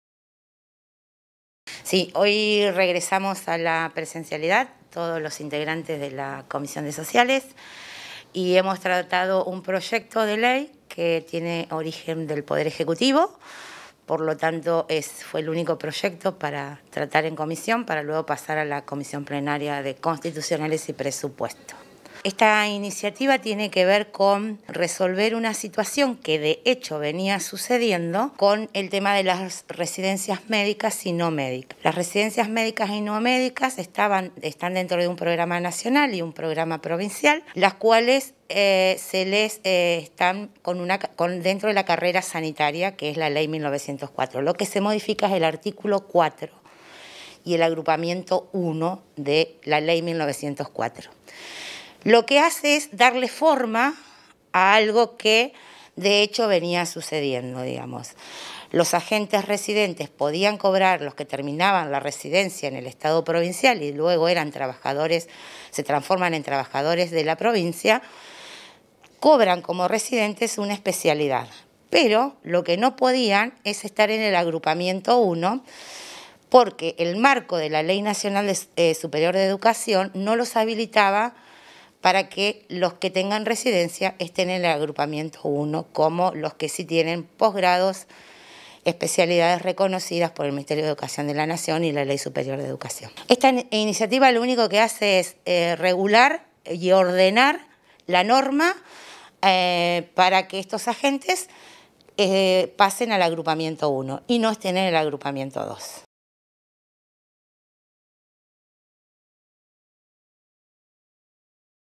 Legisladora Graciela Valdebenito- Reunión de la comisión de Asuntos Sociales.
La comisión de Sociales trató la iniciativa que plantea la incorporación al agrupamiento primero de la Carrera Técnico Profesional Sanitaria a los profesionales con residencia de salud aprobada conforme el Sistema Nacional de Residencia de la Salud.